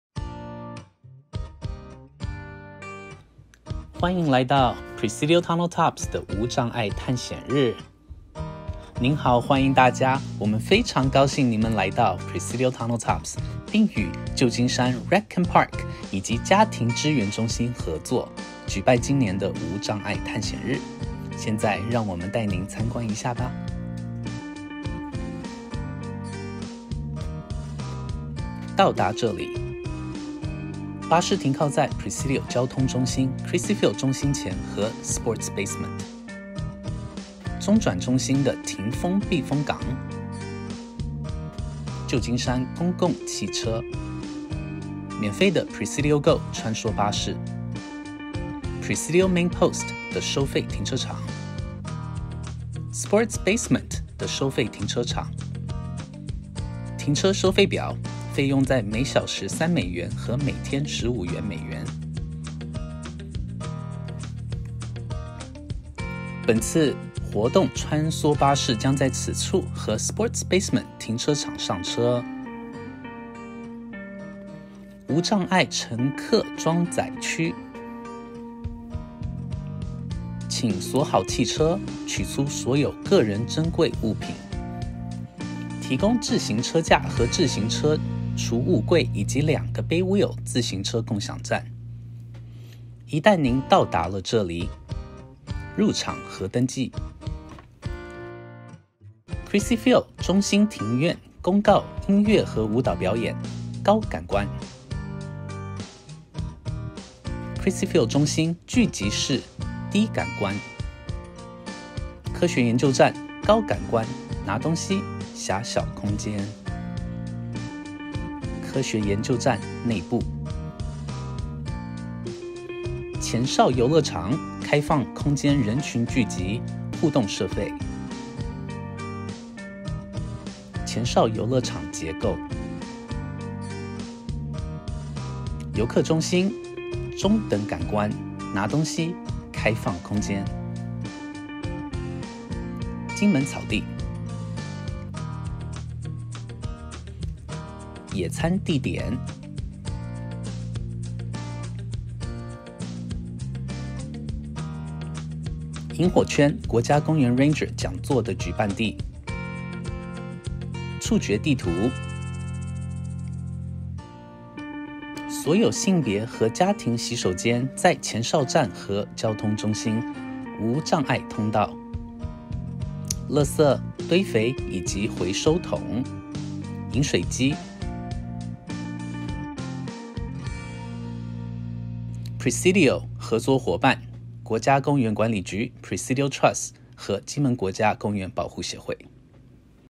Access to Adventure Day - Chinese
TRANSLATION BY: Mike Sun

When: Saturday, September 17, 2022; 12pm-3:00pm
Where: Presidio Tunnel Tops

Join Support for Families for a family fun day at the new Tunnel Tops Park in the Presidio! Learn about inclusive, recreational after-school and summer programs available in San Francisco, while exploring a brand-new accessible park.

Connect with other families and staff from Support for Families and SF’s Rec and Park Therapeutic Recreation and Inclusion Services.

Access to Adventure Day is presented by Support for Families of Children with Disabilities, SF Recreation and Parks Department, and Partnership for the Presidio.